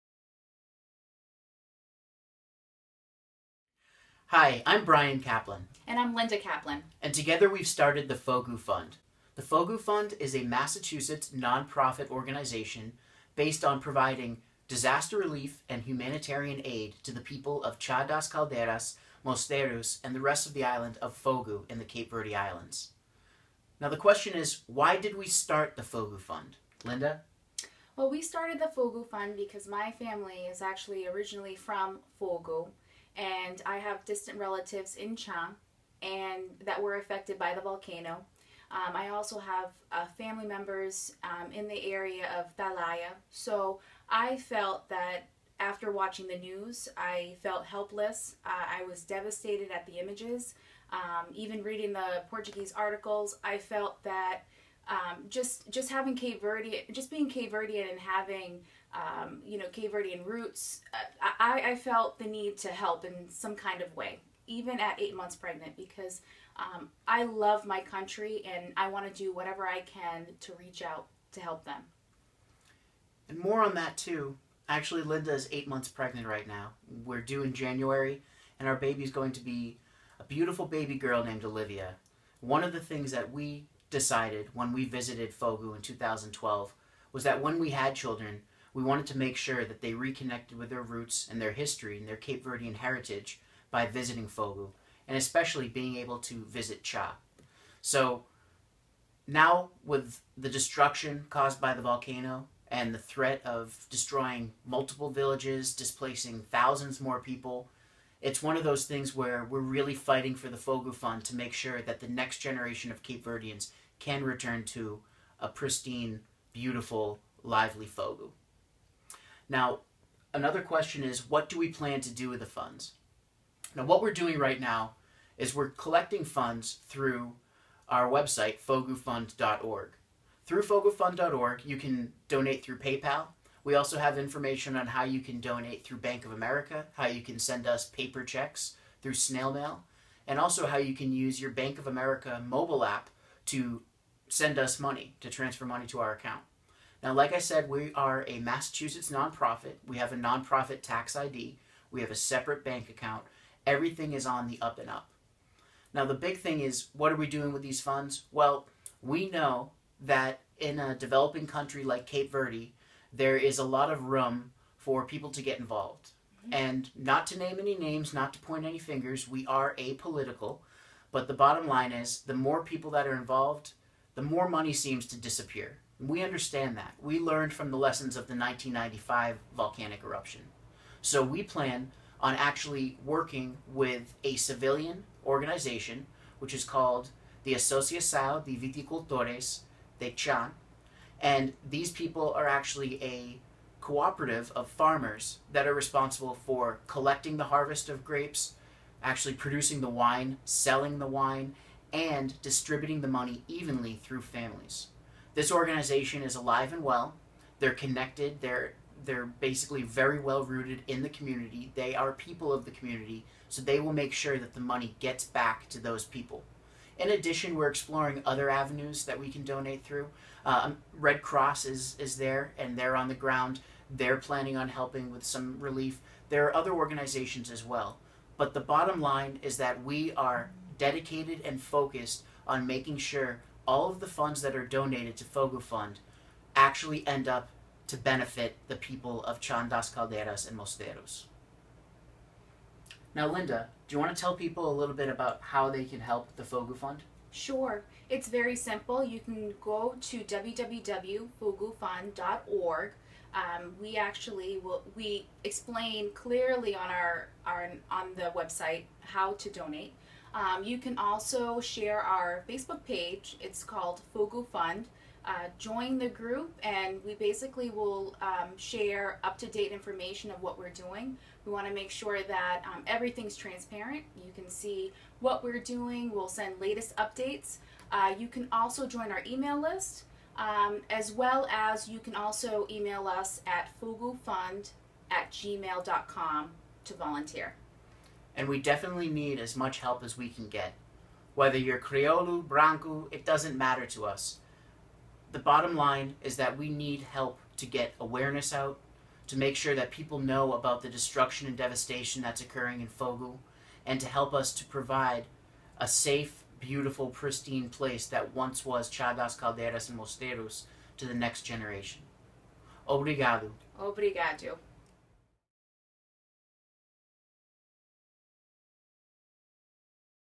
Meet The Founders of Fogo Fund
- Linda & Bryan Caplan explain why they started Fogo Fund, how you can donate, and what they plan to do with the funds raised to help the people of Cha das Caldeiras, Mosteiros, and the island of Fogo, Cape Verde.